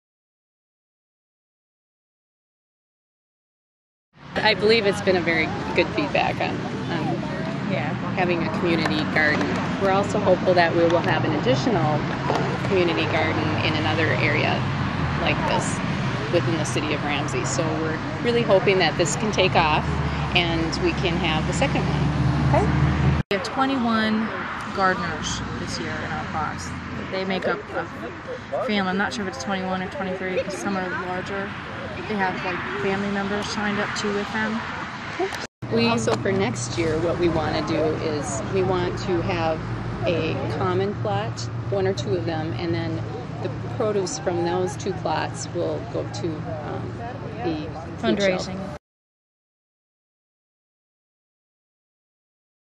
Ramsey Community Garden
The first harvest of the Ramsey Community garden.